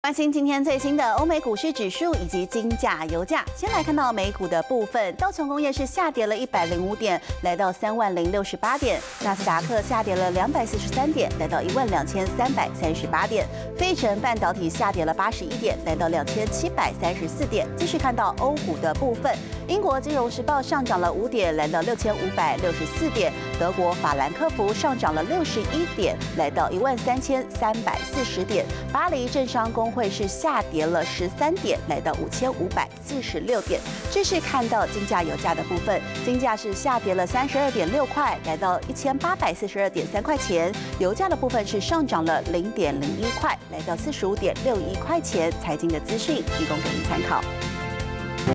國際財經最前線 歐美股市指數｜華視新聞 20201210
來關心今天最新的歐美股市指數及金價油價，美股最$線，道瓊工業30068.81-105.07，那斯達克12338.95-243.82，費城半導體2,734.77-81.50；歐股最$線，英國倫敦金融時報6564.29+5.47，德國法蘭克福13340.26+61.77，法國巴黎證商公會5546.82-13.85，國際金.油價，金價$1,842.30-$32.6，油價$45.61+$0.01。